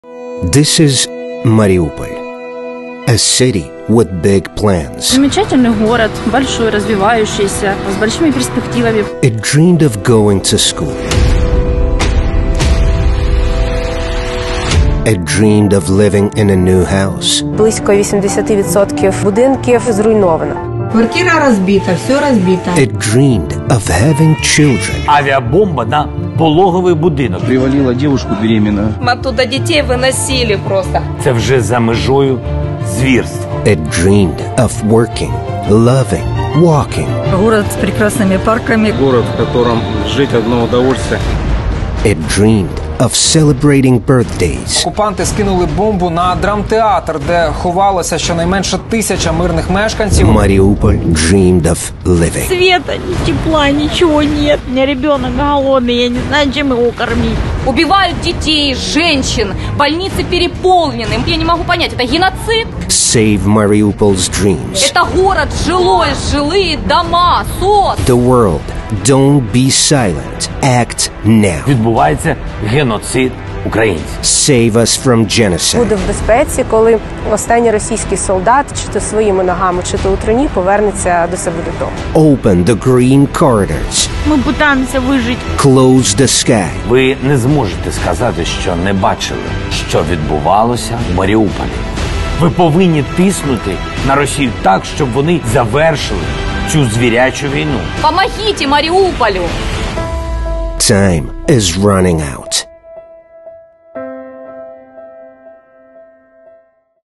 SaveMariupol ВрятуйтеМаріуполь
Майже місяць в облозі! Чотири тижні жителі Маріуполя під пекельними обстрілами. Без їжі, води, світла, тепла та зв’язку. Тисячі вже загинули. Ще десятки тисяч в смертельний небезпеці. Час діяти аби врятувати маріупольців!

Відео: телеграм-канал Маріупольської міської ради